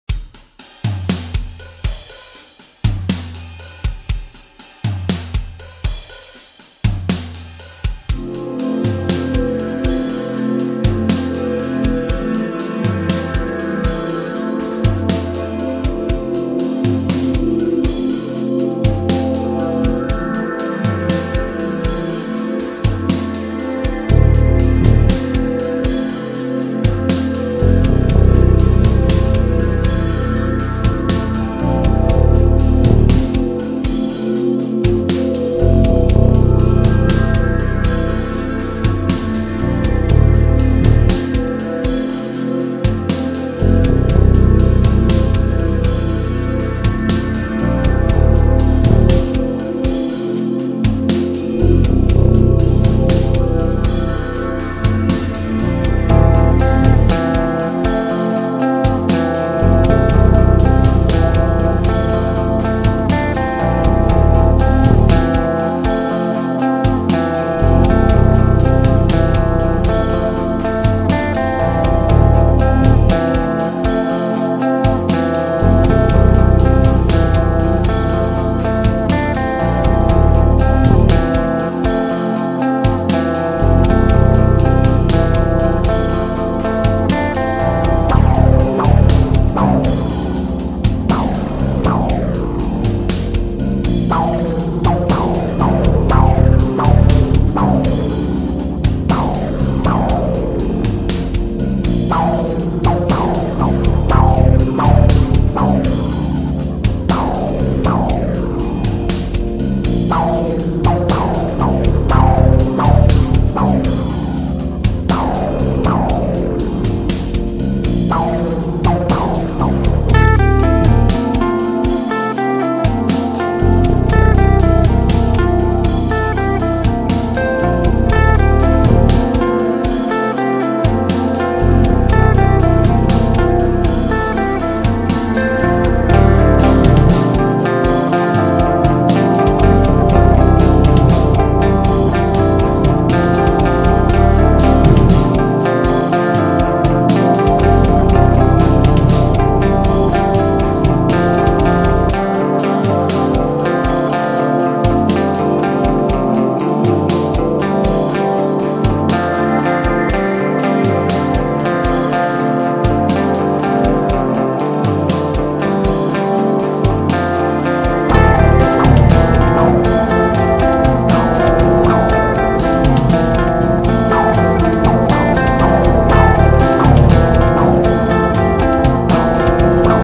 Cosmic Ghost Rider Time Lapse
AScrylic painting of the Cosmic Ghost Rider over a collage of actual comic book pages.